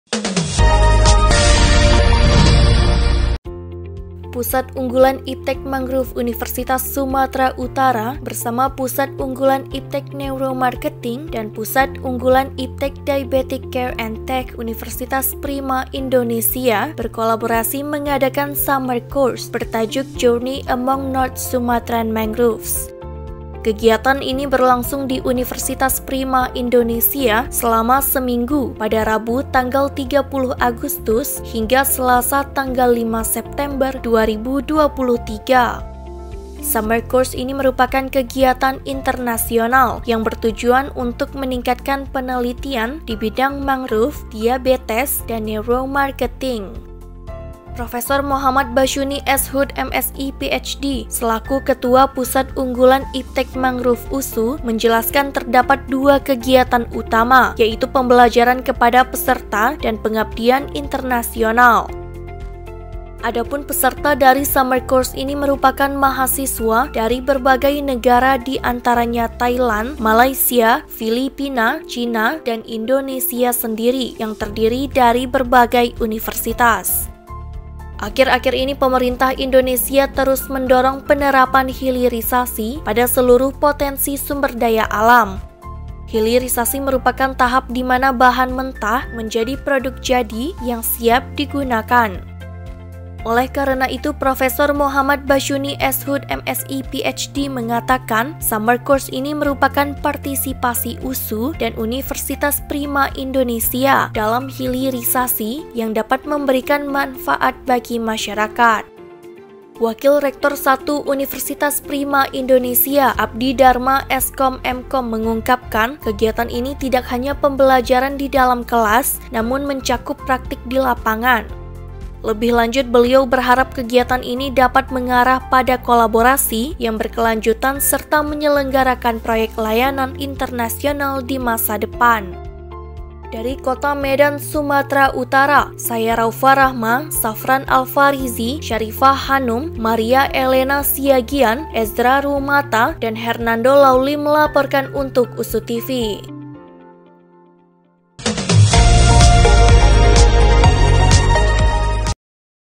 Kolaborasi PUI USU - UNPRI pada Summer Course Bahas Mangrove Sumatera Utara
Pusat Unggulan IPTEK (PUI) Mangrove Universitas Sumatera Utara (USU) bersama PUI Neuromarketing dan PUI Diabetic Care & Tech Universitas Prima Indonesia (UNPRI) berkolaborasi mengadakan Summer Course bertajuk “Journey Among North Sumatran Mangroves”. Kegiatan berlangsung di Universitas Prima Indonesia selama seminggu pada Rabu, 30 Agustus hingga Selasa, 5 September 2023.